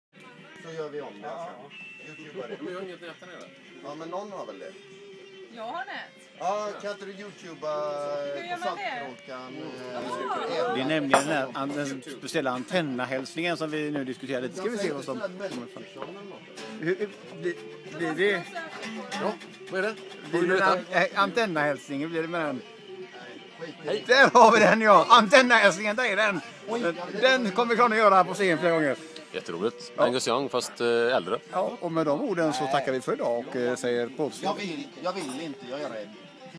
ANTENNA HÄLSNINGEN!
Bandet diskuterar Antennahälsningen innan Nalen-spelningen